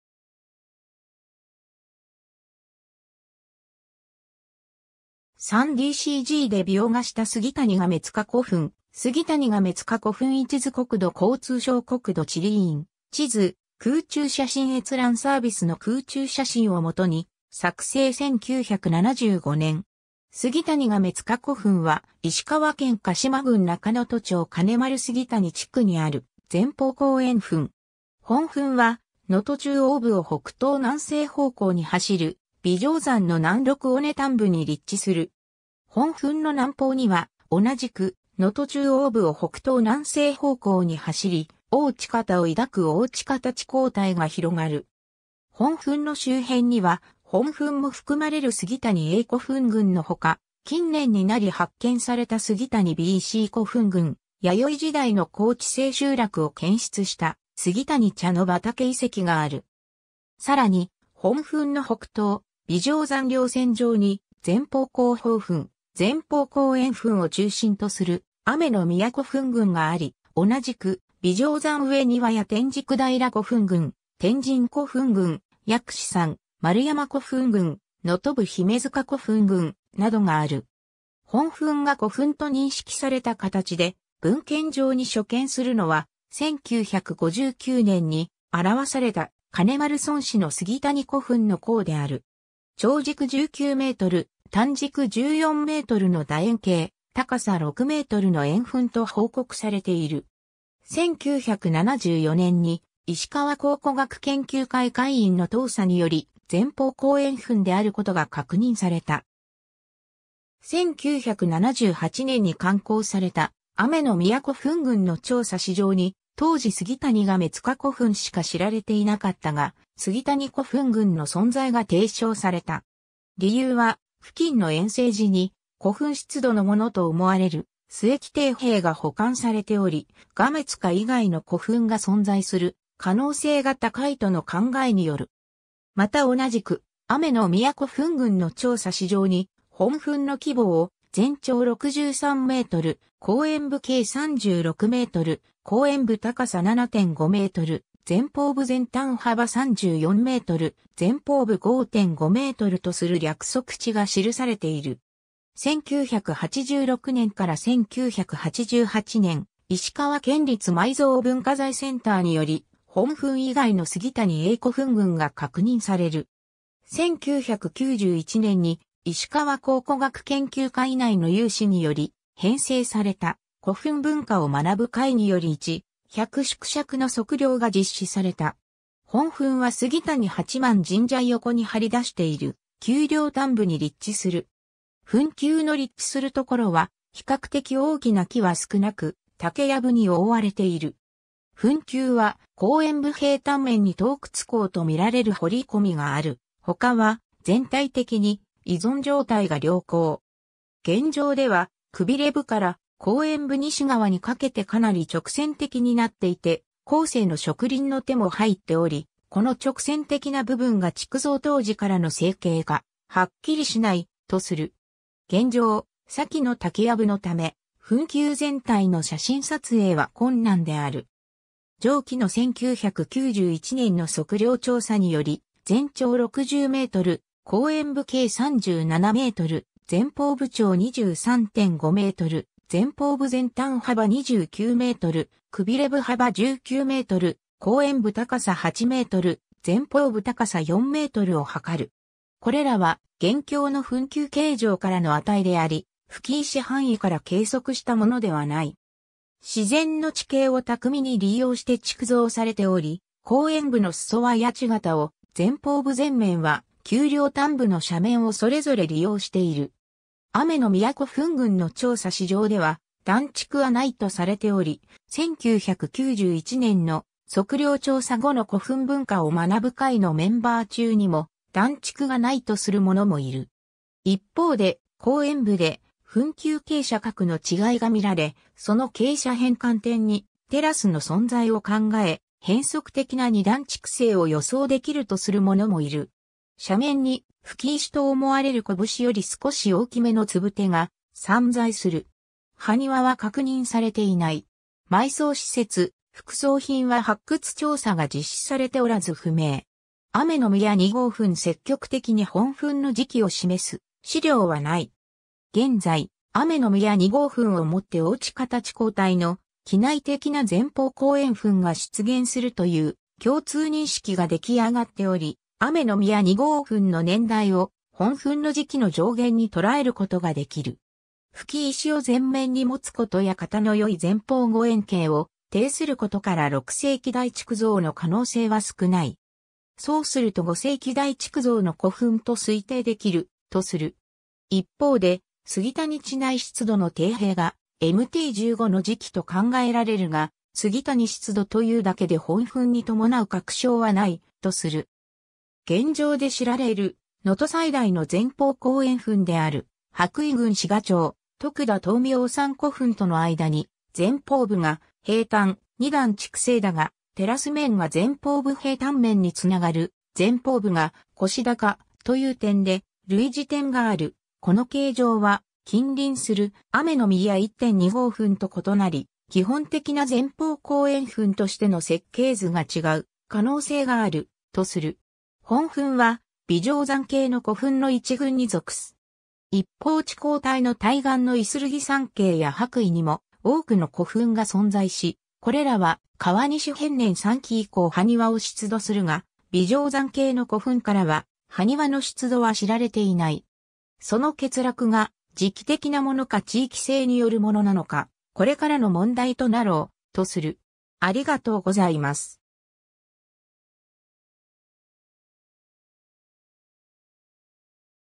杉谷ガメ塚古墳
3DCGで描画した杉谷ガメ塚古墳 杉谷ガメ塚古墳位置図 国土交通省 国土地理院 地図・空中写真閲覧サービスの空中写真を基に作成1975年。
 杉谷ガメ塚古墳（すぎたにがめづかこふん）は、石川県鹿島郡中能登町（旧鹿西町）金丸杉谷地区にある前方後円墳。
 本墳は能登中央部を北東－南西方向に走る眉丈山の南麓尾根端部に立地する。
本墳の南方には、同じく能登中央部を北東－南西方向に走り、邑知潟を抱く邑知潟地溝帯が広がる。
 本墳の周辺には本墳も含まれる杉谷A古墳群のほか、近年になり発見された杉谷B・C古墳群、弥生時代の高地性集落を検出した杉谷チャノバタケ遺跡がある。
さらに本墳の北東、眉丈山稜線上に前方後方墳・前方後円墳を中心とする雨の宮古墳群があり、同じく眉丈山上にはやテンジクダイラ古墳群、天神古墳群、薬師山・丸山古墳群、能登部姫塚古墳群、などがある。
 本墳が古墳と認識された形で文献上に初見するのは、1959年（昭和34年）に著された『金丸村史』の杉谷古墳の項である。
長軸19メートル、短軸14メートルの楕円形、高さ6メートルの円墳と報告されている。
 1974年（昭和49年）に石川考古学研究会会員の踏査により前方後円墳であることが確認された。
 1978年（昭和53年）に刊行された『雨の宮古墳群の調査』誌上に、当時杉谷ガメ塚古墳しか知られていなかったが、杉谷古墳群の存在が提唱された。
理由は、付近の圓正寺に古墳出土のものと思われる須恵器提瓶が保管されており、ガメ塚以外の古墳が存在する可能性が高いとの考えによる。
 また同じく『雨の宮古墳群の調査』誌上に、本墳の規模を、全長63メートル、後円部径36メートル、後円部高さ7.5メートル、前方部前端幅34メートル、前方部5.5メートルとする略測値が記されている。
 1986年（昭和61年）～1988年（昭和63年）、石川県立埋蔵文化財センターにより本墳以外の杉谷A古墳群が確認される。
 1991年（平成3年）に石川考古学研究会内の有志により編成された、古墳文化を学ぶ会により1/100縮尺の測量が実施された。
 本墳は杉谷八幡神社横に張り出している丘陵端部に立地する。
墳丘の立地するところは比較的大きな木は少なく、竹やぶに覆われている。
墳丘は後円部平坦面に盗掘坑と見られる掘り込みがあるほかは全体的に遺存状態が良好。
現状ではくびれ部から後円部西側にかけてかなり直線的になっていて、後世の植林の手も入っており、この直線的な部分が築造当時からの整形かはっきりしない、とする。
現状、左記の竹やぶのため、墳丘全体の写真撮影は困難である。
 上記の1991年の測量調査により、全長60メートル、後円部径37メートル（東西）、前方部長23.5メートル、前方部前端幅29メートル、くびれ部幅19メートル、後円部高さ8メートル、前方部高さ4メートルを測る。
 これらは現況の墳丘形状からの値であり、葺石範囲から計測したものではない。
 自然の地形を巧みに利用して築造されており、後円部の裾は谷地形を、前方部前面は丘陵端部の斜面をそれぞれ利用している。
『雨の宮古墳群の調査』誌上では段築はないとされており、1991年の測量調査後の古墳文化を学ぶ会のメンバー中にも段築がないとする者もいる。
一方で、後円部で墳丘傾斜角の違いが見られ（墳頂部から3メートル下がったところ）、その傾斜変換点にテラスの存在を考え、変則的な2段築成を予想できるとする者もいる。
 斜面に葺石と思われる拳より少し大きめの礫が散在する。
埴輪は確認されていない。
埋葬施設・副葬品は発掘調査が実施されておらず不明。
 雨の宮2号墳 積極的に本墳の時期を示す資料はない。
現在、雨の宮2号墳をもって邑知潟地溝帯の畿内的な前方後円墳が出現するという共通認識が出来上がっており、雨の宮2号墳の年代を本墳の時期の上限にとらえることができる。
葺石を全面に持つことや形のよい前方後円形を呈することから6世紀代築造の可能性は少ない。
そうすると5世紀代築造の古墳と推定できる、とする。
一方で、杉谷地内出土の提瓶がMT15の時期と考えられるが、杉谷出土というだけで本墳に伴う確証はない、とする。
 現状で知られる能登最大の前方後円墳である羽咋郡志賀町徳田燈明山古墳との間に、(1)前方部が平坦、(2)2段築成だがテラス面が前方部平坦面につながる、(3)前方部が腰高、という点で類似点がある、この形状は近隣する雨の宮１・２号墳と異なり基本的な前方後円墳としての設計図が違う...